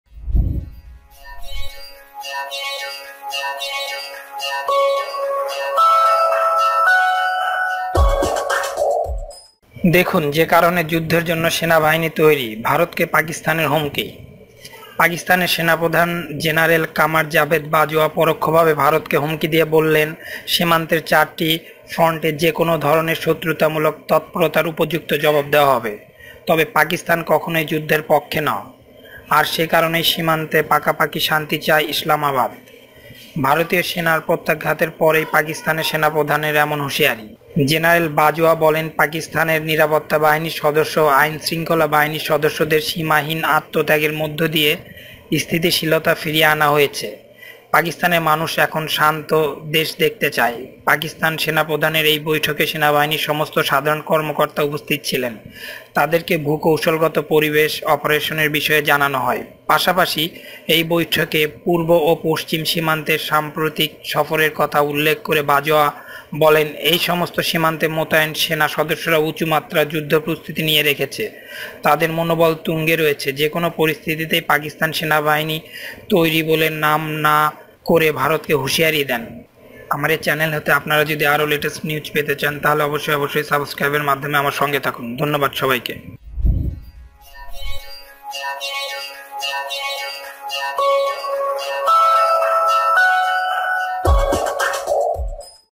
দেখুন যে কারণে যুদ্ধের জন্য সেনাবাহিনী তৈরি, ভারতকে পাকিস্তানের হুমকি
পাকিস্তানের সেনাপ্রধান জেনারেল কামার জাভেদ বাজওয়া পরোক্ষভাবে ভারতকে হুমকি দিয়ে বললেন, 'সীমান্তের চারটি ফ্রন্টে যে কোনো ধরনের শত্রুতামূলক তৎপরতার উপযুক্ত জবাব দেওয়া হবে। তবে পাকিস্তান কখনই যুদ্ধের পক্ষে নয়, আর সেই কারণে সীমান্তে পাকাপাকিভাবে শান্তি চায় ইসলামাবাদ। ' ভারতীয় সেনার প্রত্যাঘাতের পরেই পাকিস্তানের সেনাপ্রধানের এমন হুঁশিয়ারি।

জেনারেল বাজওয়া বলেন, পাকিস্তানের নিরাপত্তা বাহিনীর সদস্য ও আইন-শৃঙ্খলা বাহিনীর সদস্যদের সীমাহীন আত্মত্যাগের মধ্যদিয়ে স্থিতিশীলতা ফিরিয়ে আনা হয়েছে। পাকিস্তানের মানুষ একটি শান্ত দেশ দেখতে চায়। পাকিস্তান সেনাপ্রধানের এই বৈঠকে সেনাবাহিনীর সমস্ত সাধারণ কর্মকর্তা উপস্থিত ছিলেন। তাদেরকে ভূ-কৌশলগত পরিবেশ, অপারেশনের বিষয়ে জানানো হয়। পাশাপাশি, এই বৈঠকে পূর্ব ও পশ্চিম সীমান্তে সাম্প্রতিক সফরের কথা উল্লেখ করে বাজওয়া বলেন,  এই সমস্ত সীমান্তে মোতায়েন সেনা সদস্যরা উঁচু মাত্রার যুদ্ধ-প্রস্তুতি নিয়ে রেখেছে। তাদের মনোবল তুঙ্গে রয়েছে।  যেকোনও পরিস্থিতিতেই পাকিস্তান সেনাবাহিনী তৈরি বলে নাম না করে ভারতকে হুঁশিয়ারি দেন। সূত্র: কলকাতা টুয়েন্টিফোর।